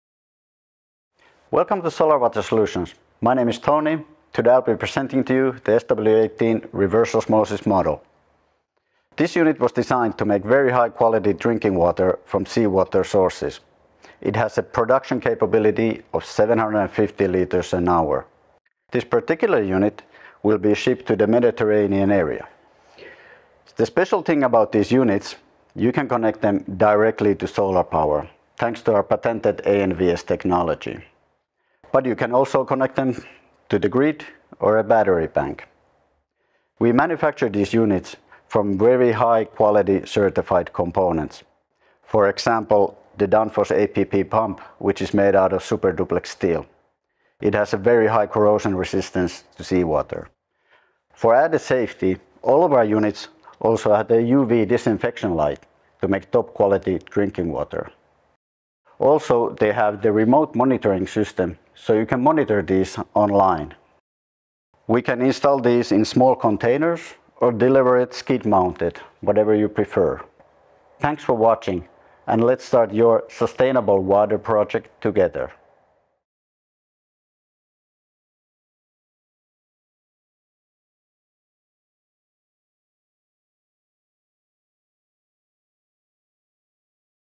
Solar-Powered Seawater Desalination | Sustainable Water for Remote Locations
🌊 Instant Sustainable Water | Solar-Powered Desalination ☀️💧

🔹 Struggling with clean water access? Our solar-powered desalination units provide clean, affordable water—off-grid, grid-tied, or hybrid. Watch as our Head of Manufacturing, Toni, presents this cutting-edge desalination unit!

🔹 Ideal for:
✔ Remote communities – Secure a clean, local water source without relying on infrastructure.
✔ Natural disaster response – Rapid deployment for emergency drinking water supply.
✔ Agriculture – Irrigation-ready desalination for off-grid farms.
✔ Hospitality industry – Reliable, eco-friendly water supply for hotels & resorts.
✔ Off-grid & unstable grid areas – Independent or hybrid water access solutions.

🚰 Why This System?
✔ 100% Solar-Powered Opportunity – No fuel, no emissions, no high energy costs.
✔ Fresh Water from the Ocean – Converts seawater into clean, safe drinking water.
✔ Scalable & Low-Maintenance – Reliable, cost-effective, and designed for efficiency.

📌 Key Technical Highlights of the SW18 seawater model:
💧 Water Production: Up to 6,000 liters/day with direct solar power
🔋 Energy Efficient: 5.5 kWh/m³ specific energy consumption
📡 Smart Monitoring: DrivePro® remote control for easy operation

🌎 With 300+ successful projects worldwide, we help customers achieve water independence with renewable energy.